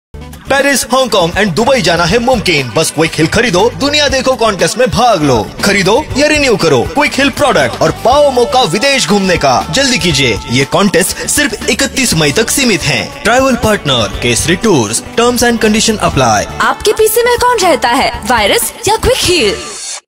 Quick Heal Kharido Duniya Dekho contest
Buy or renew a Quick Heal product and get assured gifts and a chance to win a trip to Paris, Hong Kong or Dubai.